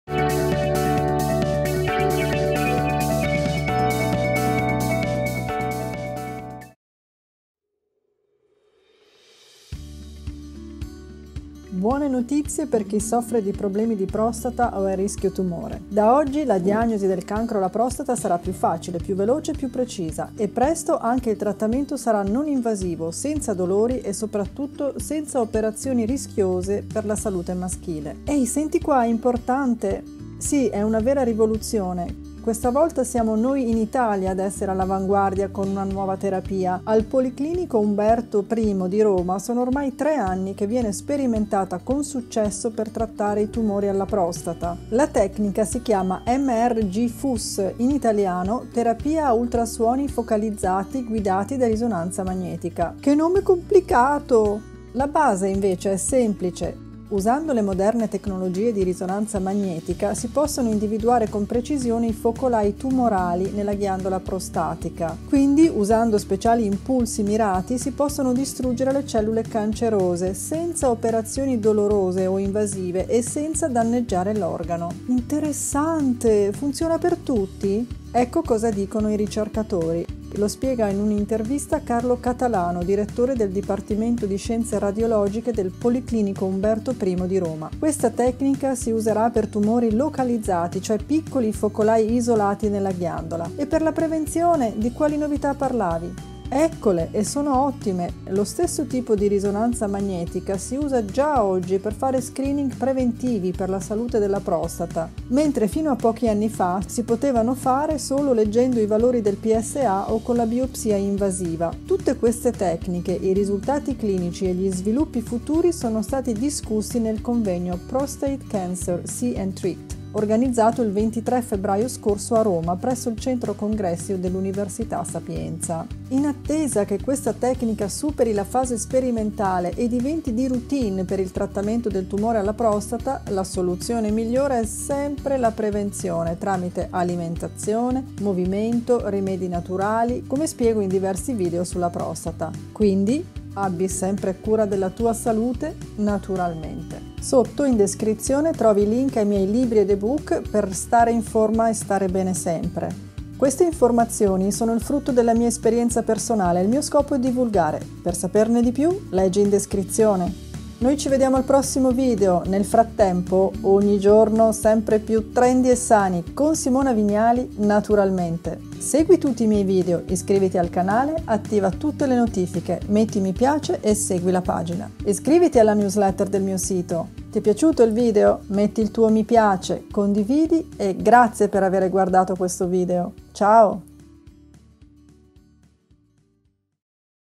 PROSTATA CURA DEFINITIVA: terapia anti TUMORE PROSTATA NON INVASIVA. Ricerca Medica
Ciao amici! Oggi condivido con voi una notizia molto positiva per la lotta e la cura del tumore alla prostata!

Nota bene

Simona Vignali